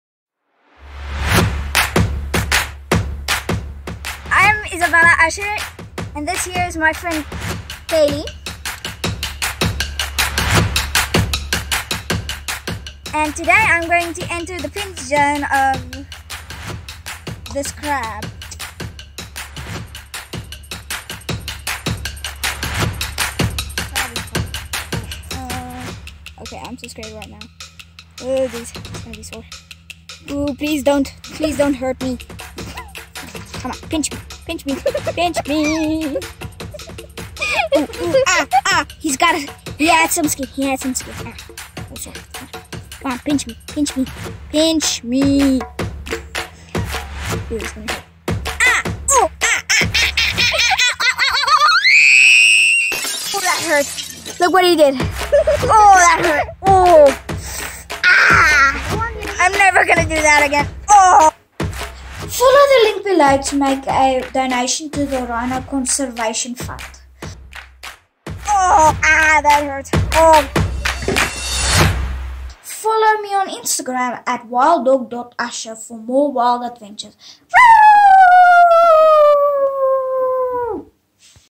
PINCHED BY A TINY CRAB
The Pinch Zone Challenge:

To raise awareness and money for Rhino Conservation, Izabella puts her finger on the line.
You can follow Wild Dog Usher on Instagram: @wilddog.usher for more wild fun!

The crab was not harmed in anyway during this spontaneous WildDog Usher video. Izabella is also fine.

Help Izabella raise money for the Rhino Conservation Fund by clicking on the above link and donating to save the Rhino's!